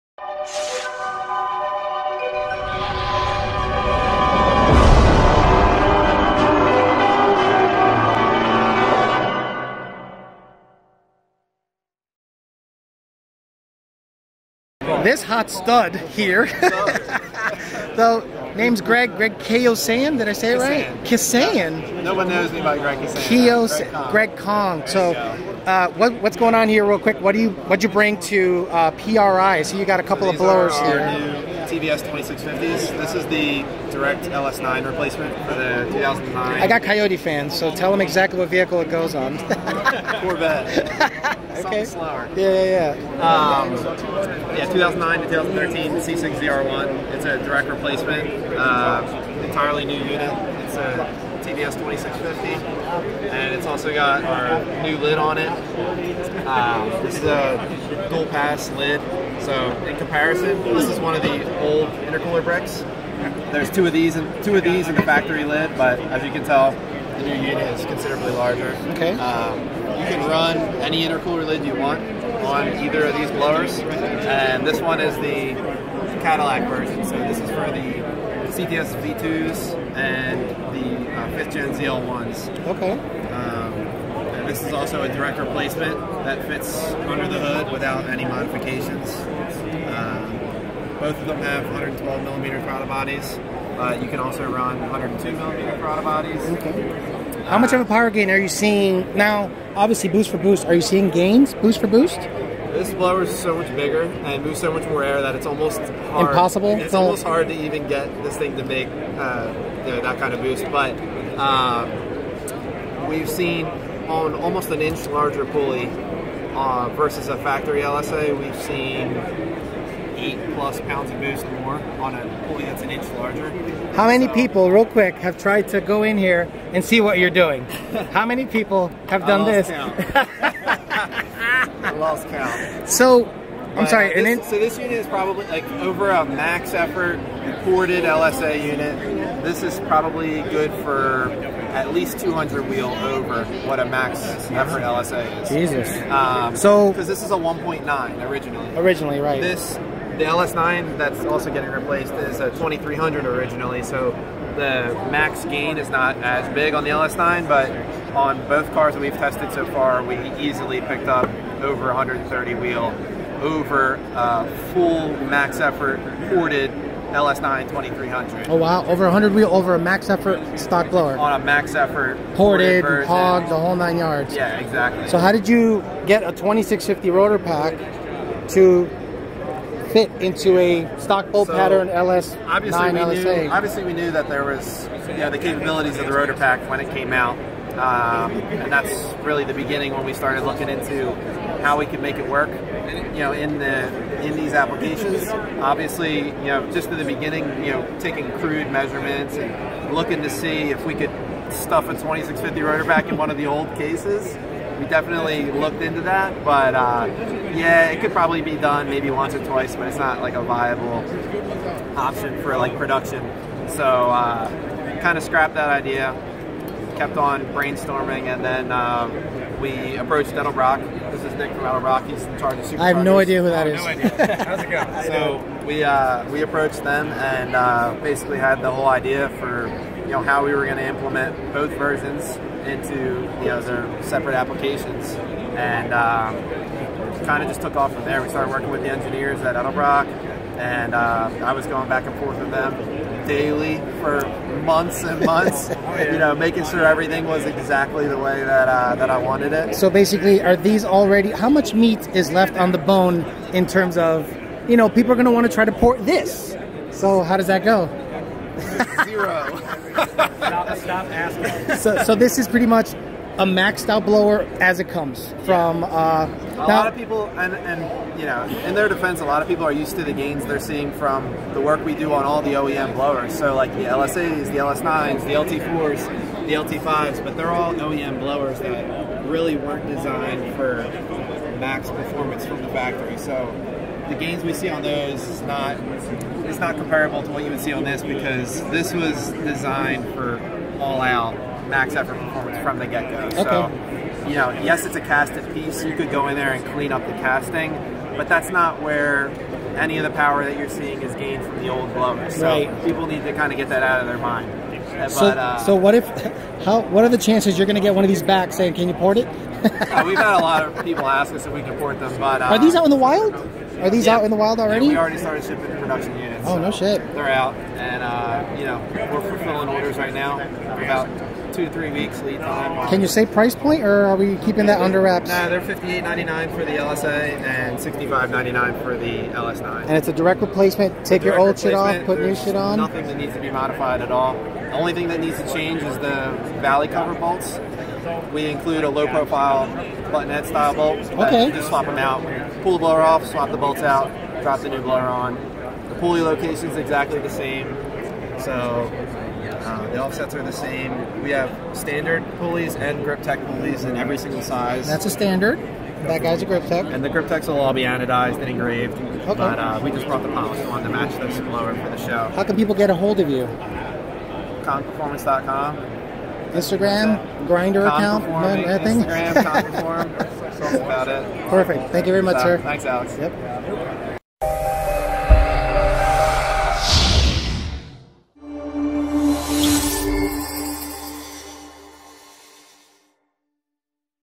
Kong performance TVS 2650 LS and LT series supercharger upgrade
Blowers are my thing and Ex Lund racing tuner Greg "Kong" has been making great progress porting and upgrading superchargers of all types , this is his newest product, a 2650 direct bolt on blowers for CTSV and ZR1 C6 corvettes and many other applications.